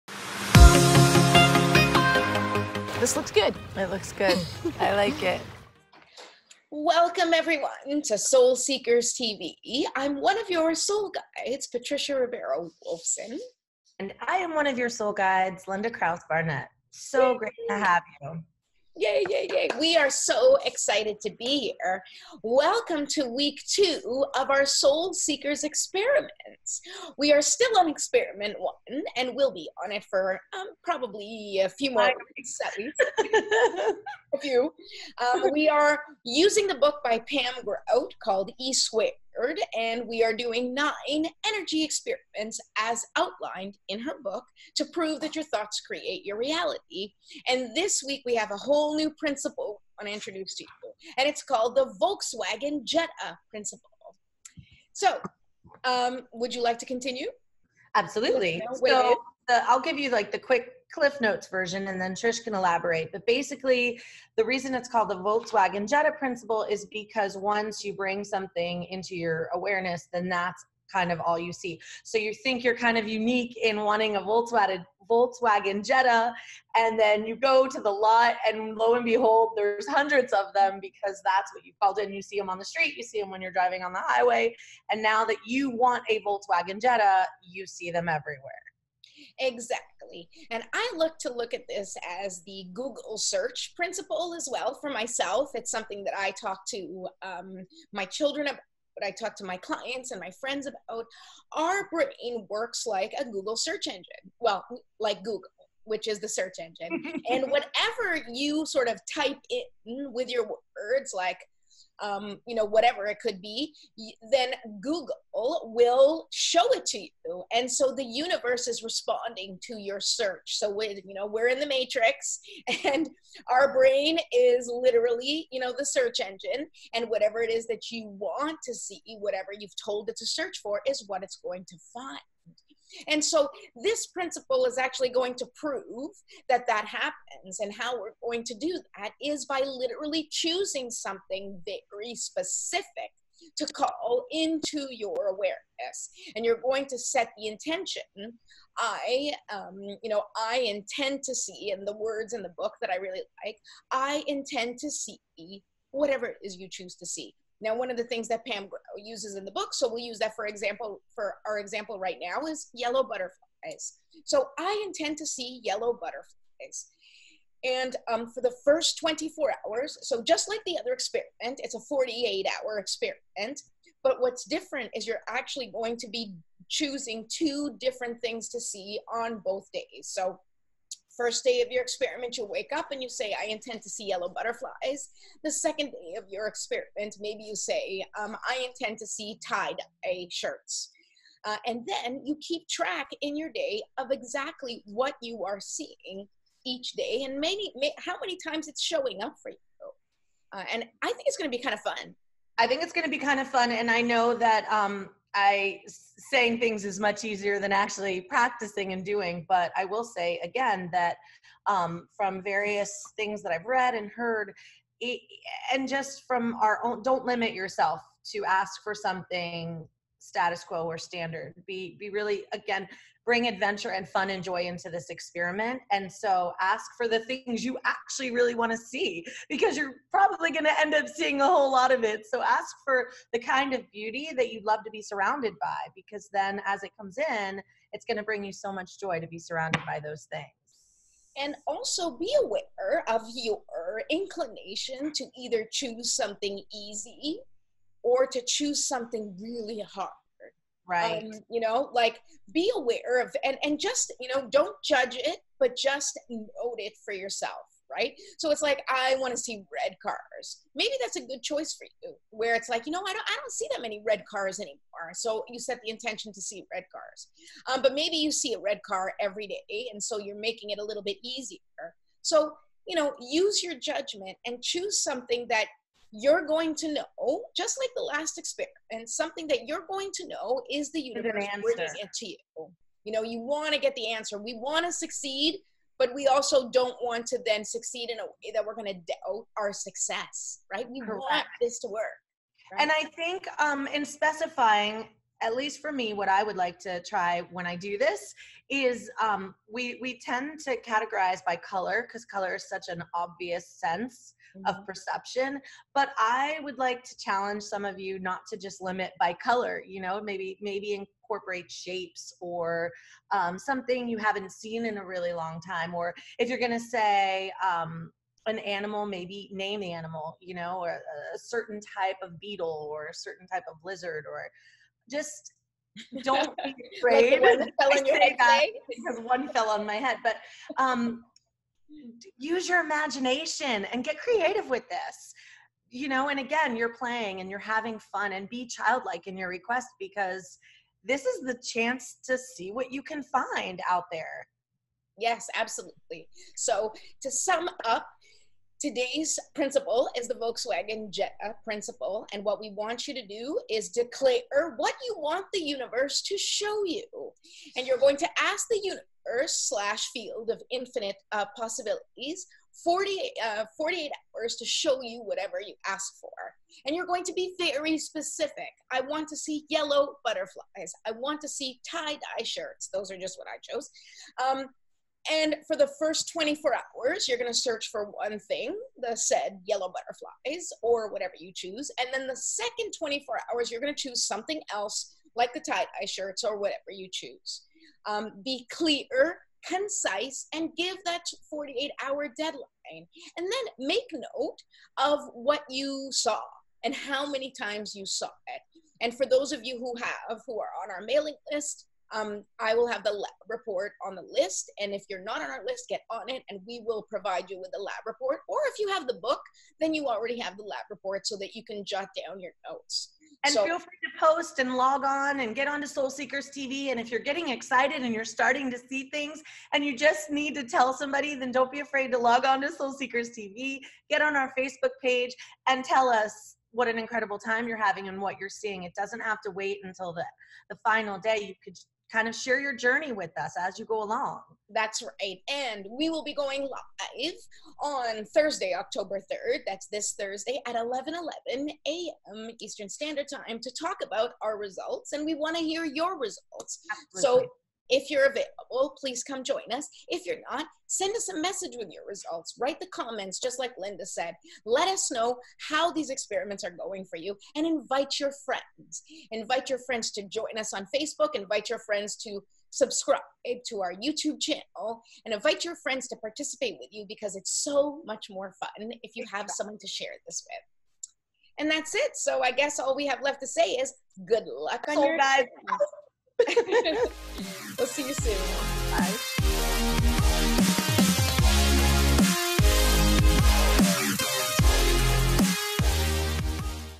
SSTV E-Squared - The Volkswagen Jetta Principle
Welcome to week 2 of our Soul Seekers Experiments.

We are doing NINE Energy Experiments to PROVE that your thoughts create your reality as outlined in the book E-Squared by @Pamgrout.

THE VOLKSWAGEN JETTA PRINCIPLE

"You impact the field and draw from it according to your beliefs and expectations" - Pam Grout

In this 48 hour experiment, you will declare what you want the Universe to show you. Choose one thing such as "sunset beige cars" as per the book's suggestion for the first 24 hours and then "yellow butterflies" for the next 24 hours.

If you have not joined our experiment yet, it's NOT too late, sign up

Please like, follow and share with anyone that you think might benefit from this experience.